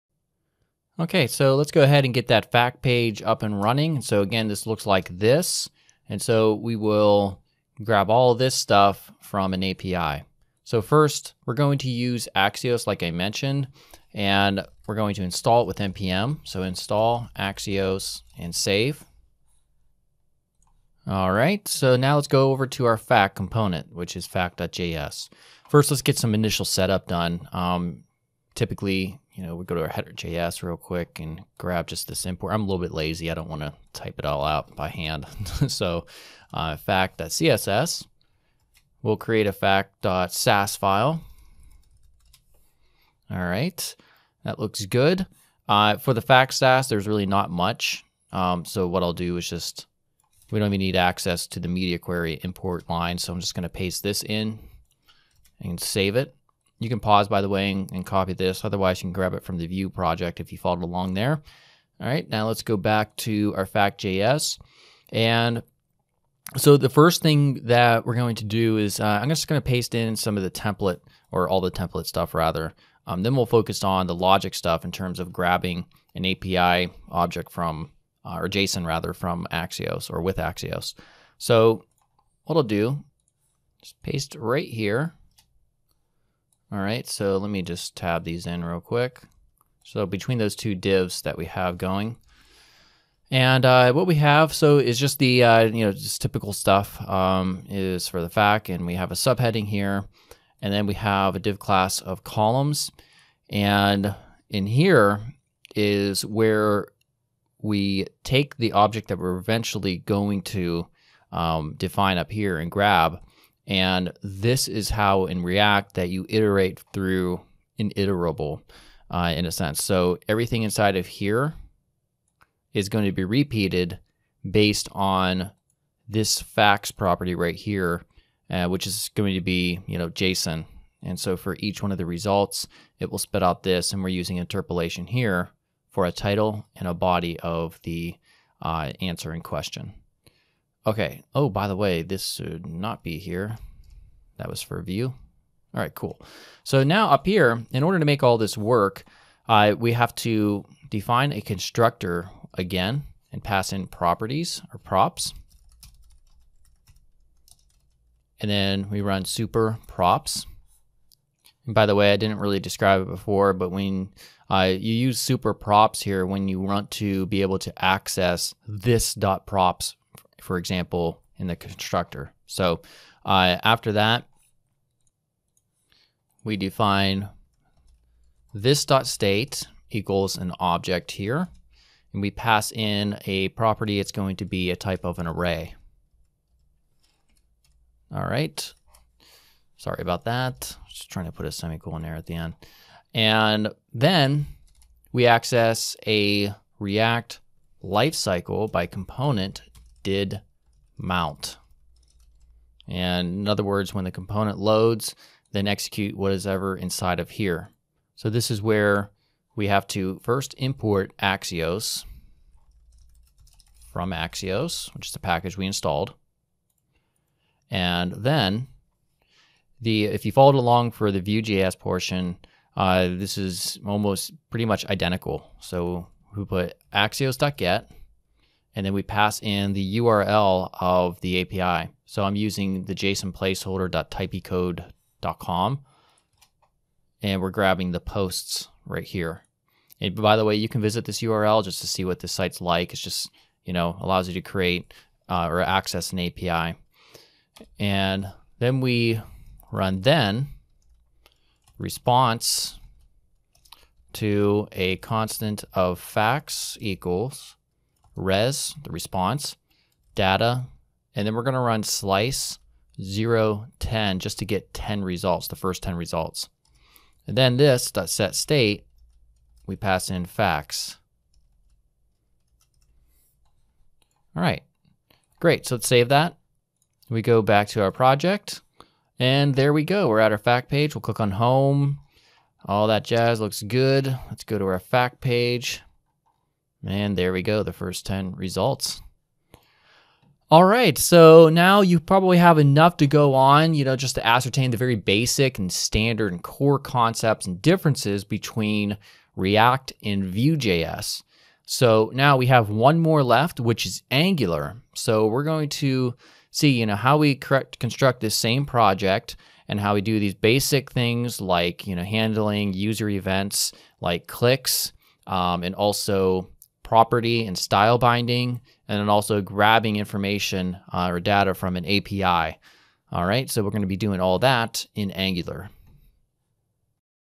React js FAQ Page #24
In this video, we will structure the FAQ page and use Axios to fetch data from an API.